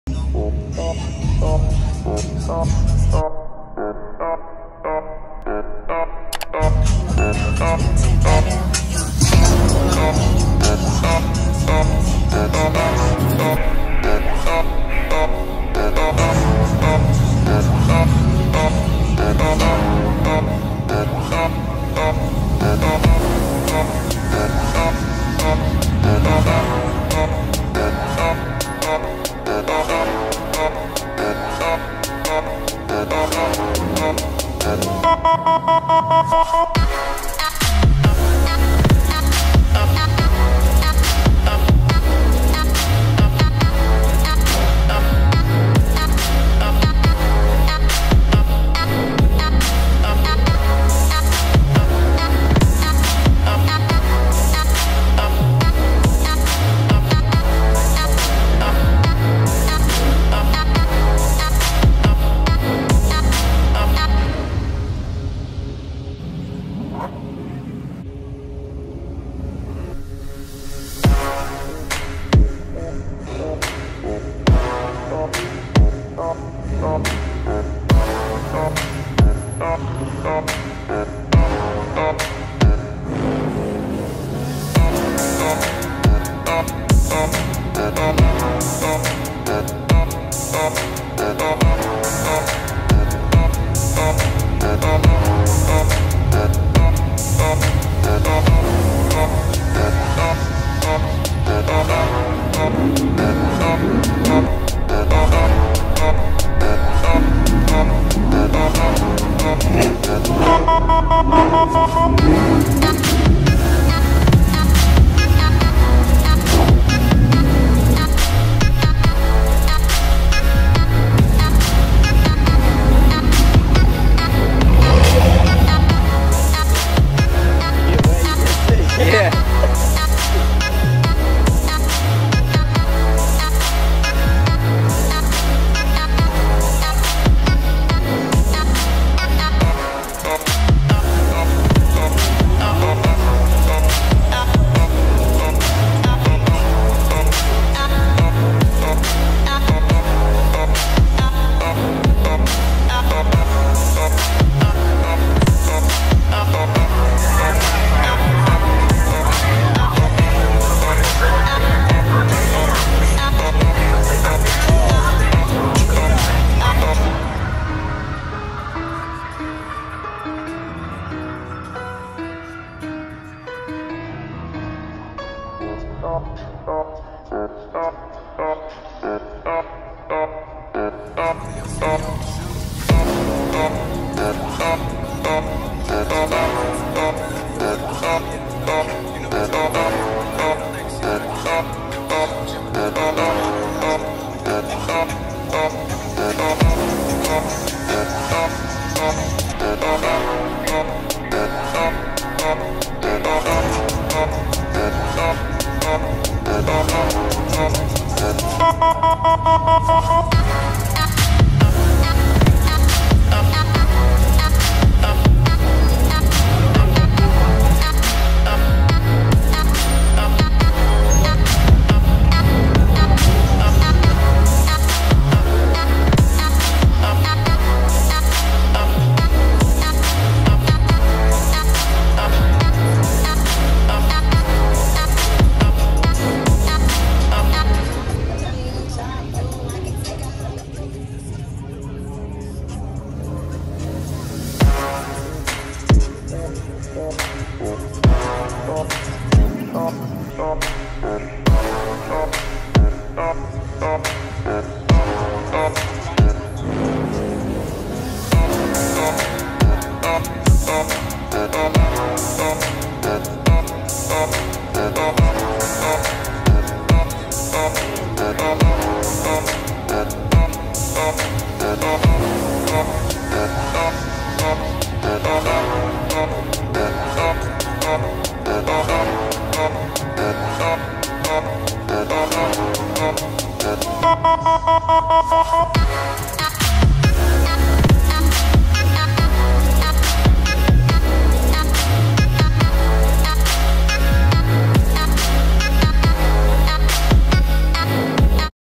Jax wax Gastonia street madness meet❗️
Short clip of some cars from street madness in Jax Wax Gastonia 👍🏽